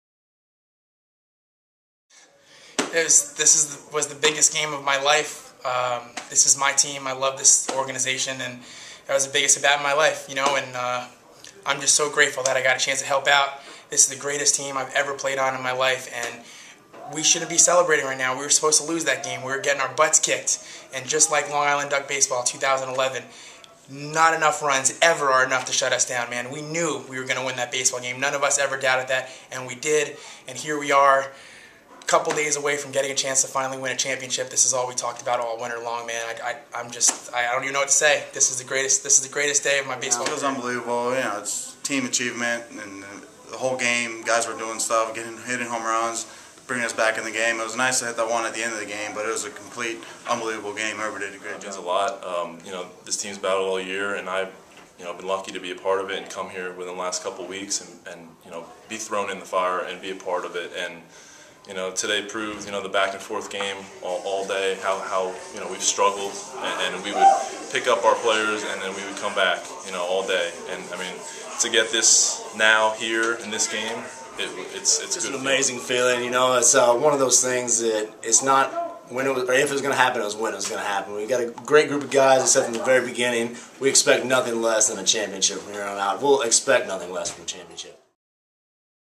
Players Talk About Liberty Division Title 9-25-11
Ray Navarrete, Freddie Thon, Jon Hunton, and Bubbie Buzachero talk about the winning the Liberty Division Championship in 2011!